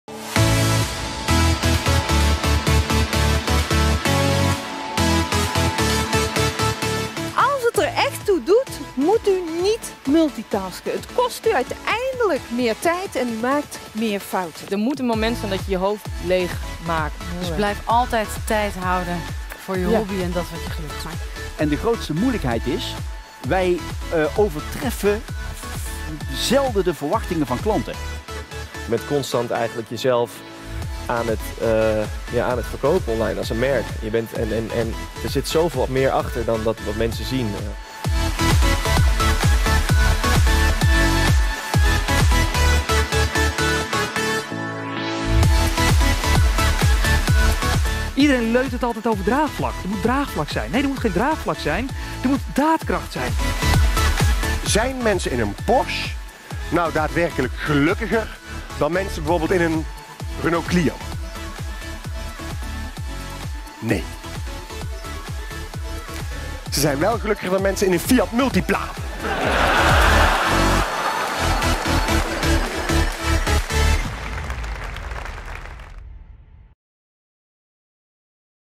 Aftermovie - De Ondernemers Show 2018
Sprekers:
Merel Westrik, Margriet Sitskoorn, José Woldring, Jos Burgers, Julian Jordan, Juul Manders, Bas van der Veldt, Guido Weijers.

Datum:
03 juli 2018

Locatie:
Afas Live | Amsterdam

Aantal deelnemers:
1800

Opdrachtgever:
RTL Z